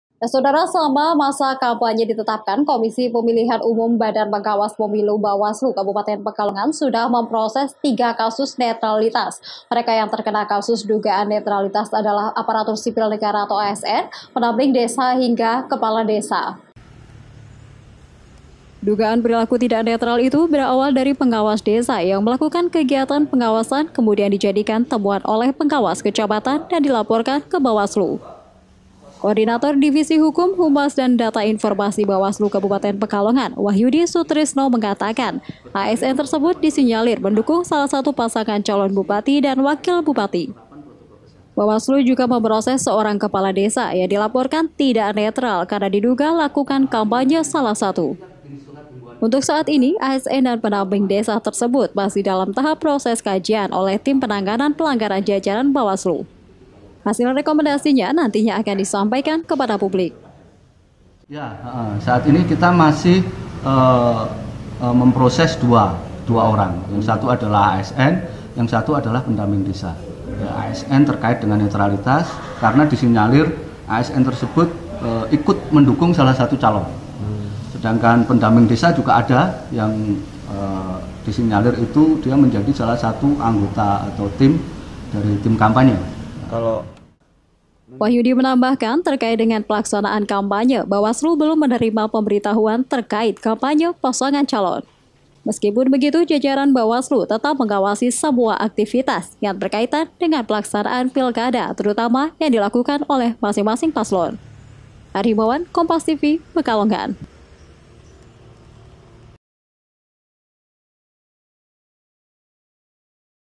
Bawaslu Tangani Tiga Kasus Dugaan Ketidaknetralan
PEKALONGAN, KOMPAS.TV - Dugaan perilaku tidak netral itu berawal dari pengawas desa yang melakukan kegiatan pengawasan kemudian dijadikan temuan oleh pengawas kecamatan dan dilaporkan ke Bawaslu.

Koordinator divisi hukum, Humas, dan data Informasi Bawaslu Kabupaten Pekalongan, Wahyudi Sutrisno mengatakan ASN tersebut disinyalir mendukung salah satu pasangan calon Bupati dan Wakil Bupati. Bawaslu juga memproses seorang kepala desa yang dilaporkan tidak netral karena diduga lakukan kampanye salah satu.

Untuk saat ini ASN dan pendamping desa tersebut masih dalam tahap proses kajian oleh tim penanganan pelanggaran jajaran Bawaslu. Hasil rekomendasinya nantinya akan disampaikan kepada publik.

Wahyudi menambahkan, terkait dengan pelaksanaan kampanye,  Bawaslu belum menerima pemberitahuan terkait kampanye pasangan calon. Meskipun begitu jajaran Bawaslu tetap mengawasi semua aktifitas yang berkaitan dengan pelaksanaan pilkada terutama yang dilakukan oleh masing-masing paslon.